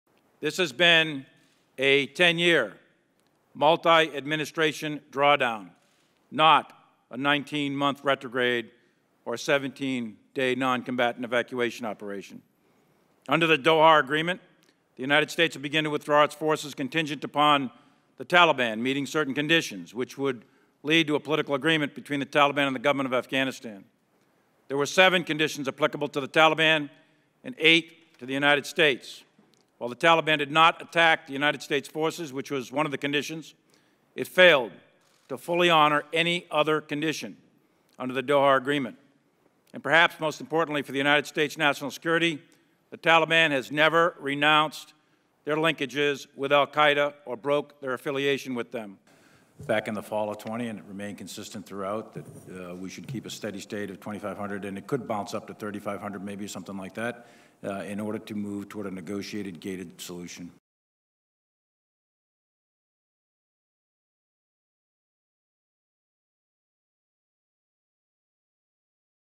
Taliban never cut ties with al-Qaeda, says US General
طالبان نے القاعدہ سے وابستگی کبھی نہیں توڑی: امریکی جنرل